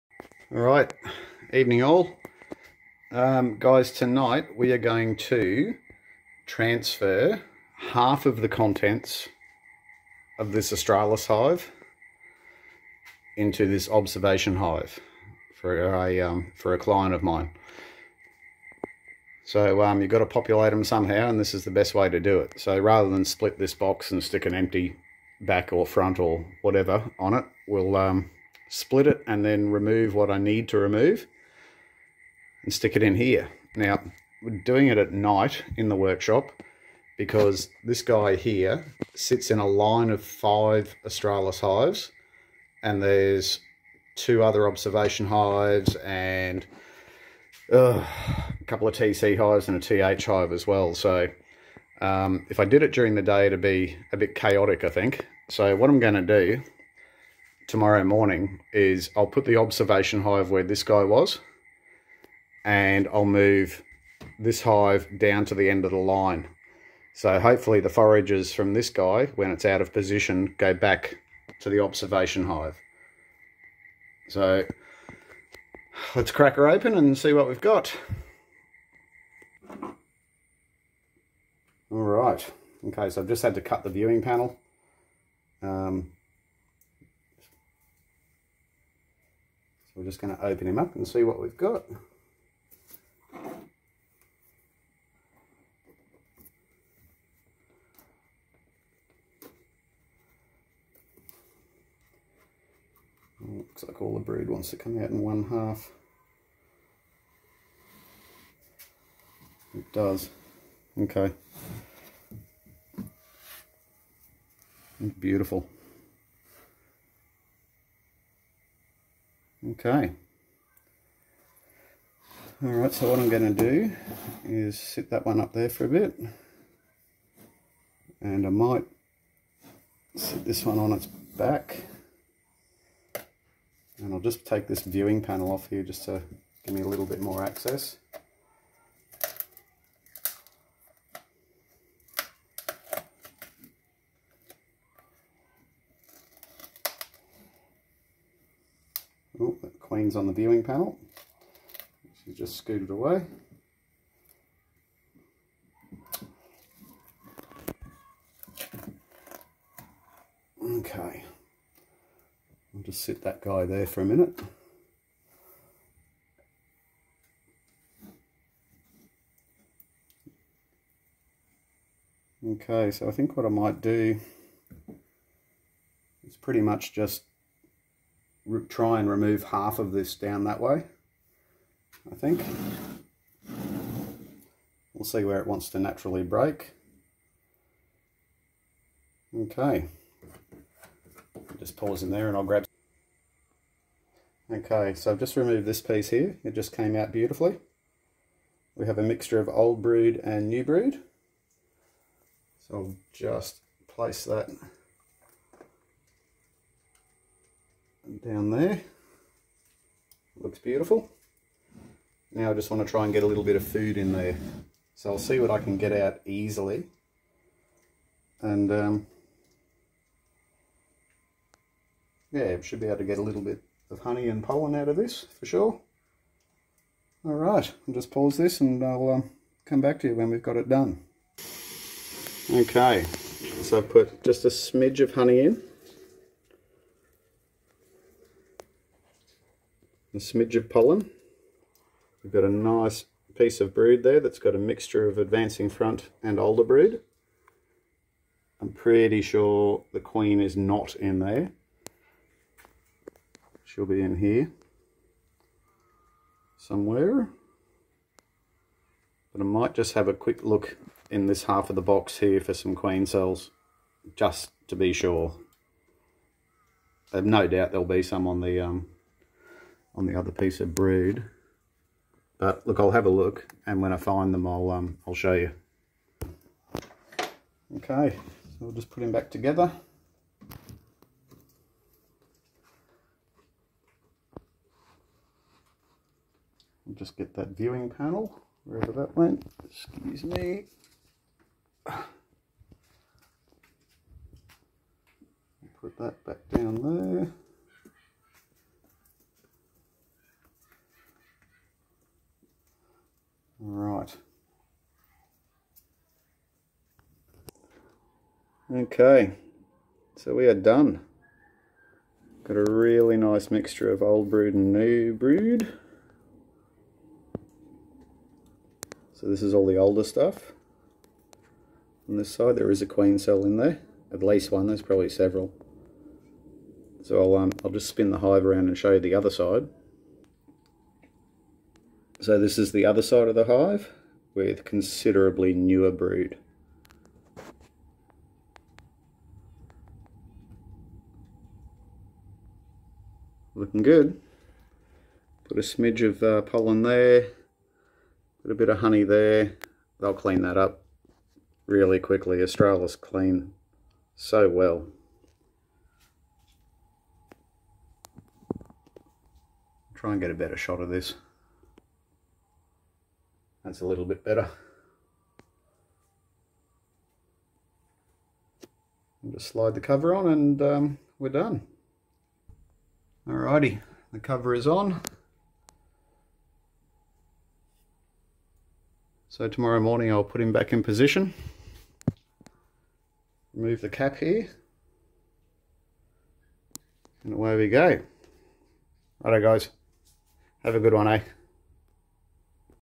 Populating one of our Australis Observation Hives
This is pretty much the only way to populate a Observation Hive with Austroplebeia Australis. Basically, this a broodlift from an existing colony and has proved to be extremely successful in the past